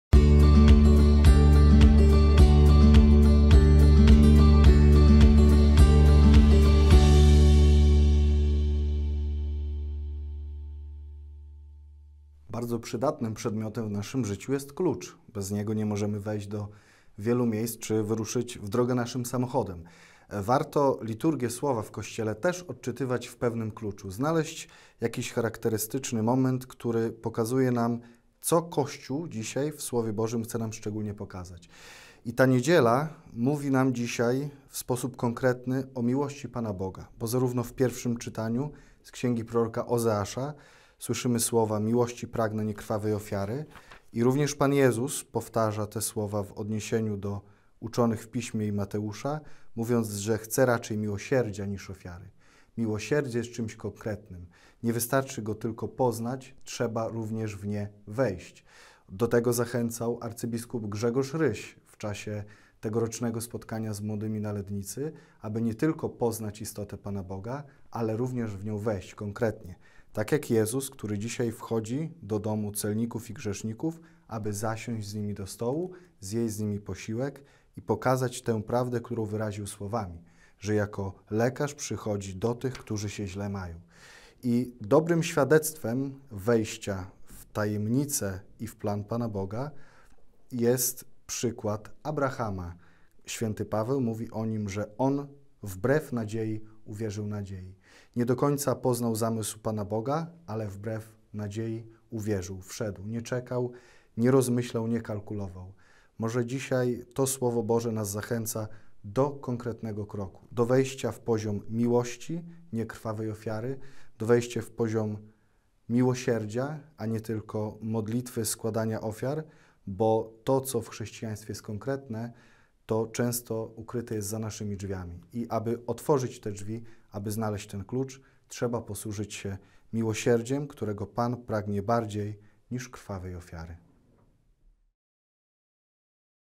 oBOGUznadBUGA [213] 11.06.2023
Komentarz KsMichałSiduniak na 10. Niedzielę zwykłą (Rok A)